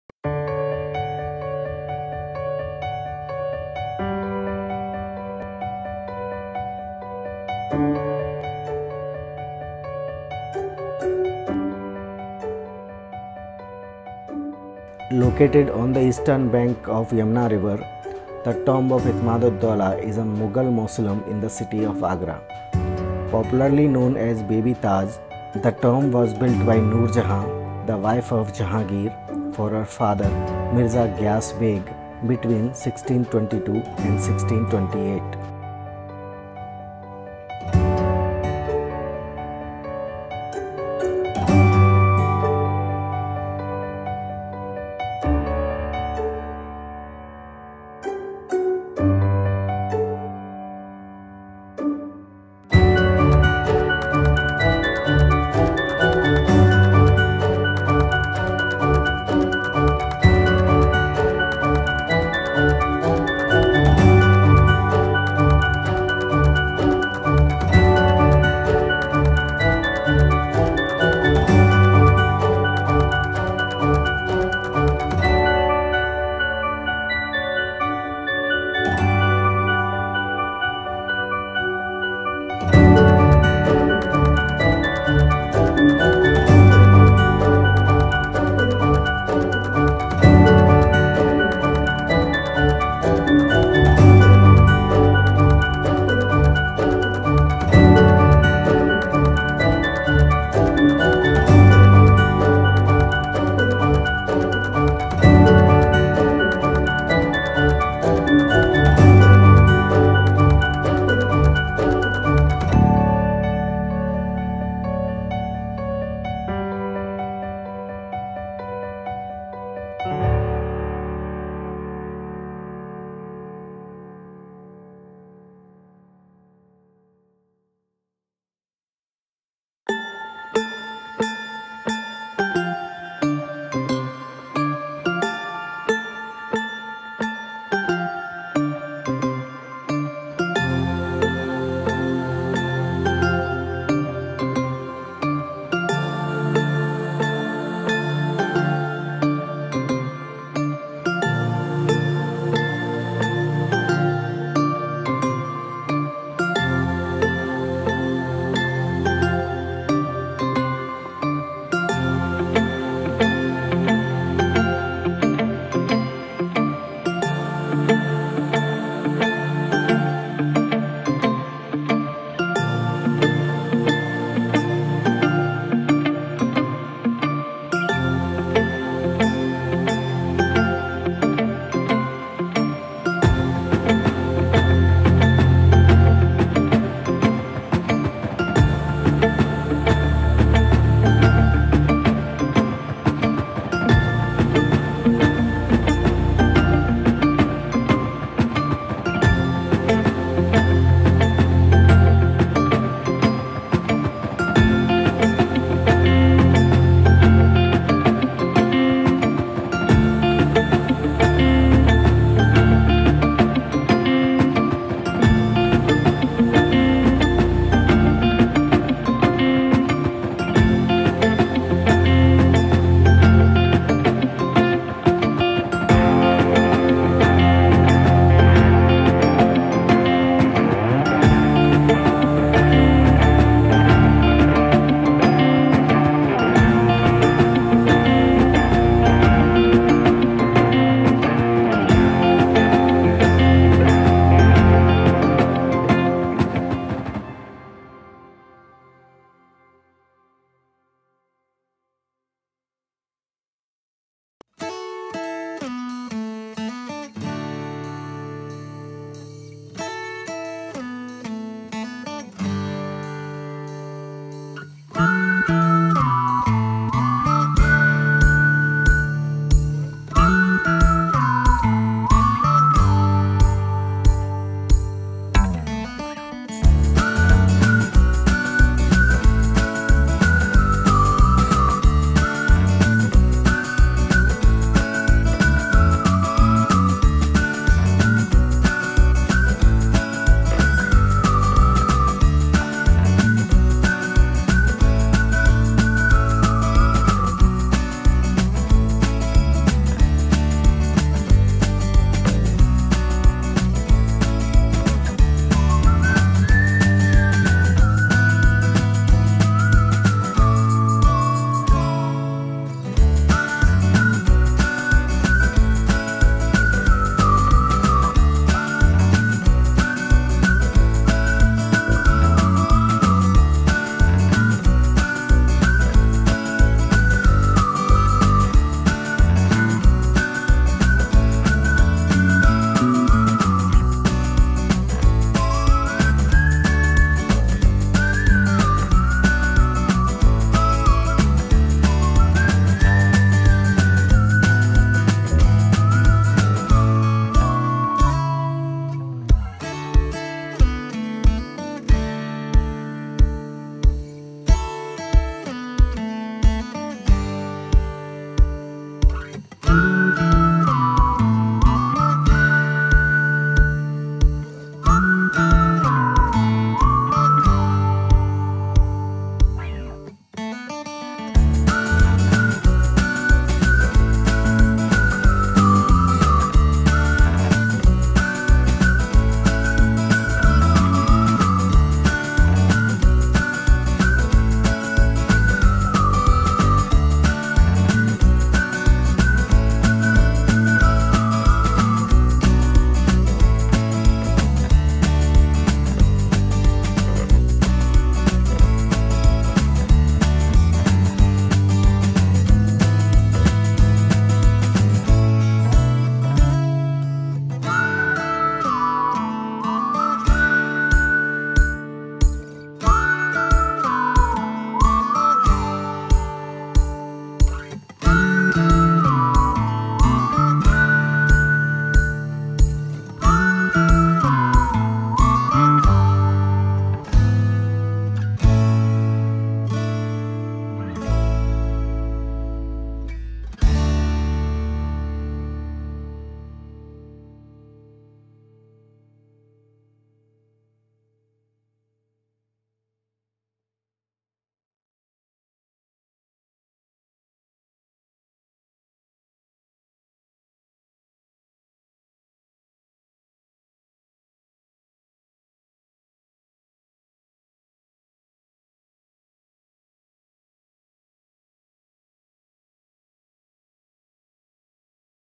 Itmad Ud Daulah Tomb Popularly Known as The Baby Taj Mahal
Located on the eastern bank of the Yamuna River, the tomb of Itmad-ud-Daulah is a Mughal mausoleum in the city of Agra. Popularly known as Baby Taj Mahal, The tomb was built by Nur Jahan, the wife of Jahangir, for her father Mirza Ghiyas Beg  between 1622 and 1628 in history. Itmad Ud Daulah Tomb is one of the most visited places in Agra throughout the year.

Itmad Ud Daulah Timings: The Baby Taj is open from 6 AM in the morning till 6:00 PM in the evening.